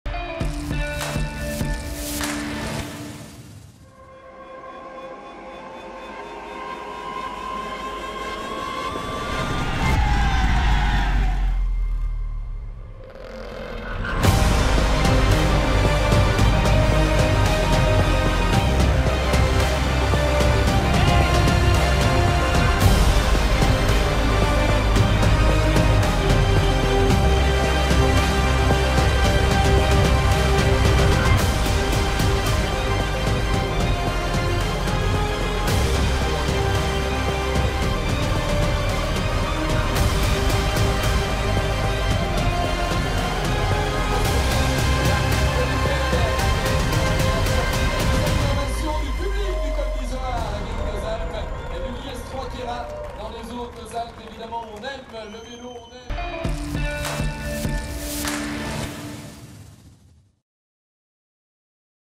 The ŠKODA green jersey minute - Stage 18 - Tour de France 2017
The 2017 Tour de France starts for the 1st time from Düsseldorf in Germany.
From Saturday 1st of July to Sunday 23rd of July 2017, the 104th Tour de France includes 21 stages for a total length of 3 521 kilometers.

Stage 18 - (Briançon / Izoard)